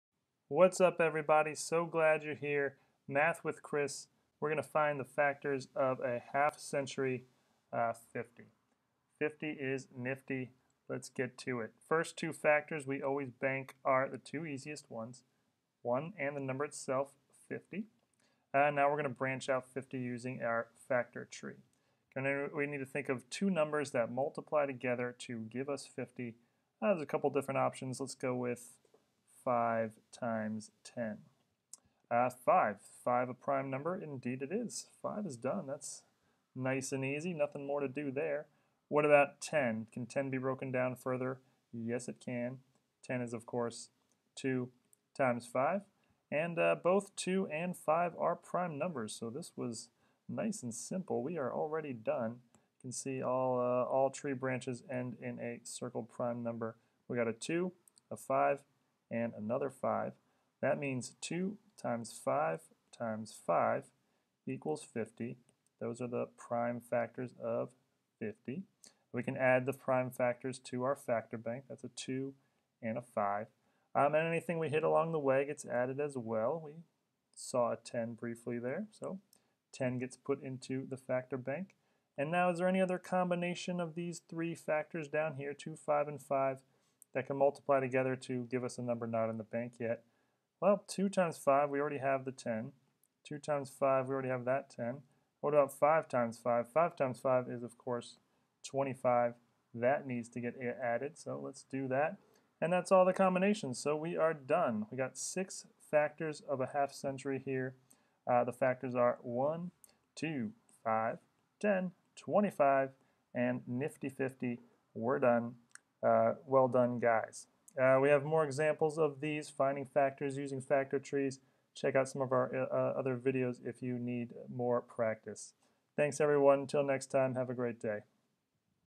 Finding the Factors of 50
Determining the factors of 50 using a factor tree.  Going through all the steps involved, branching out the tree, finding all the prime factors, and using them to multiply together to find any factors we may have missed along the way.  Just like addition, multiplication, subtraction, division, fractions, and many more, this is an important math skill to have for any level!